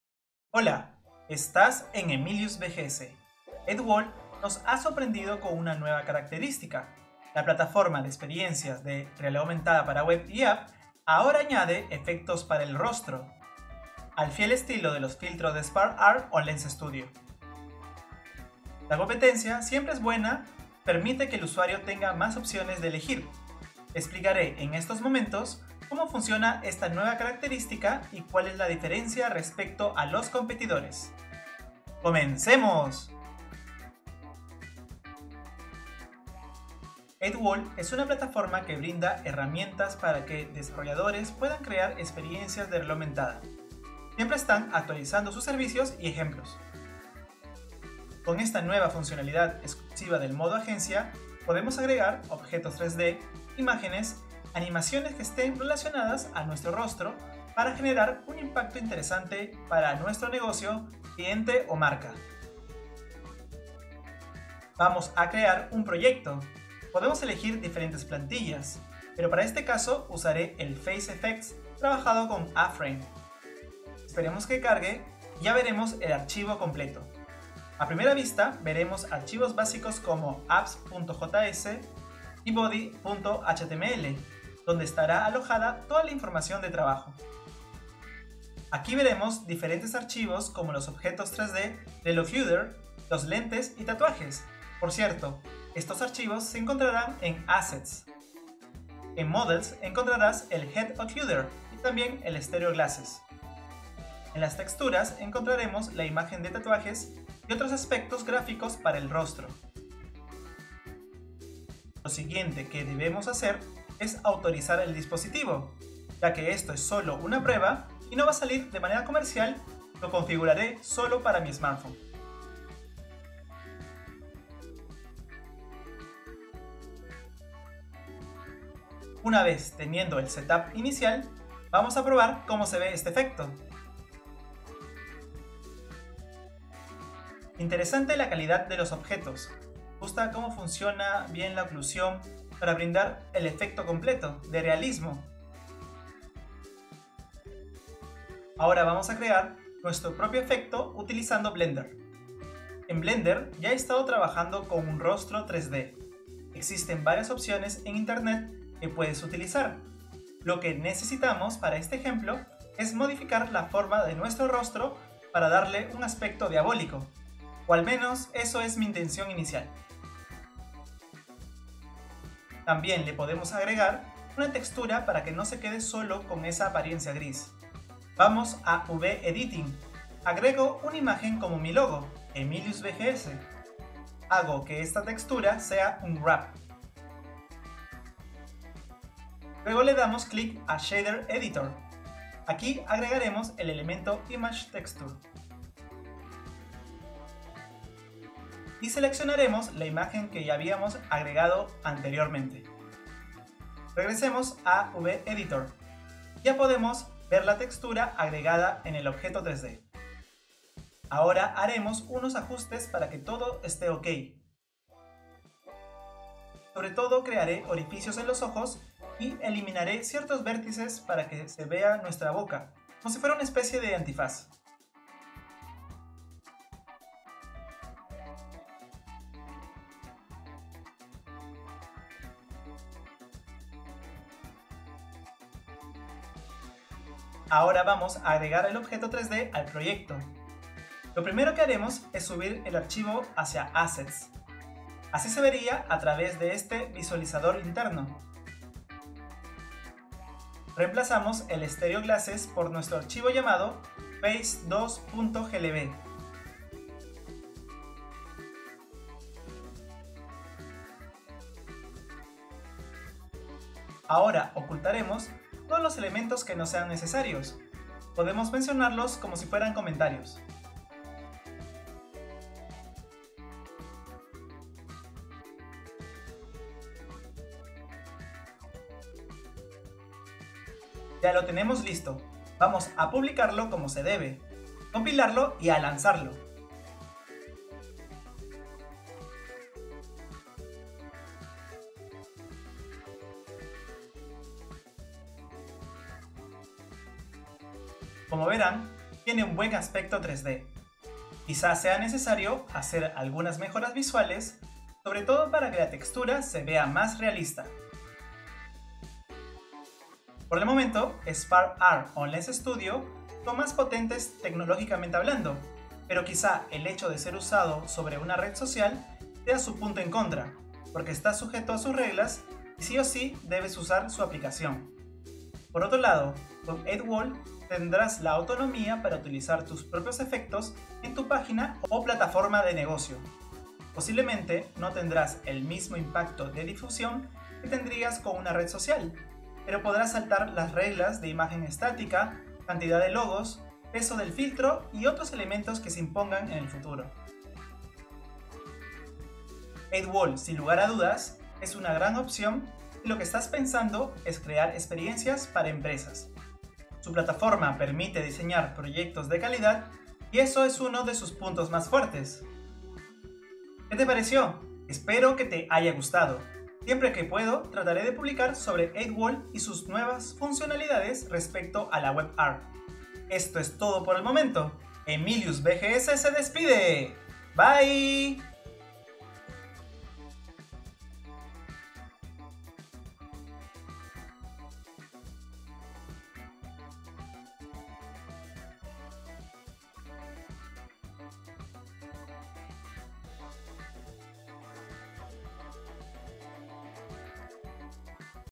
How to create a AR Face Effect with 8thwall? WebAR
Hola! En este video hablaremos sobre la nueva funcionalidad de 8th wall para realizar efectos de realidad aumentada para el rostro. AR Effect es la nueva propuesta que tiene esta empresa para desarrollo WebAR. ¿Será mejor que Spark AR  o Lens Studio?
Hi! In this video we will talk about the new functionality of 8th wall to perform augmented reality effects for the face. AR Effect is the new proposal that this company has for WebAR development. Will it be better than Spark AR or Lens Studio?

Mi VR / AR software y hardware:

⚡ Oculus Quest
⚡ Insta 360 one X
⚡ Cardboard
⚡ RiftCat
⚡ Aryzon
⚡ Zapbox
⚡ Merge Cube